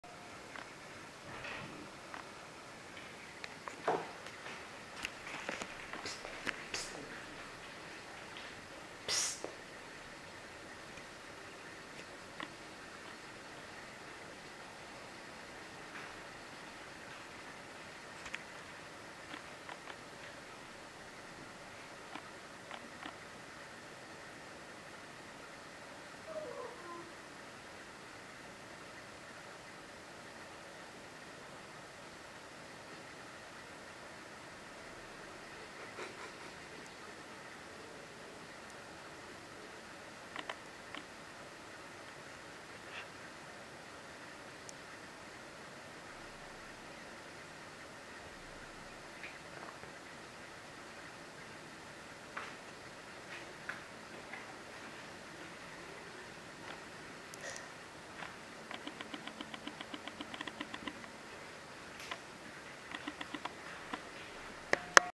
Occupy mission
Raffi decided to sleep on the big cat tree - to Amber's amazement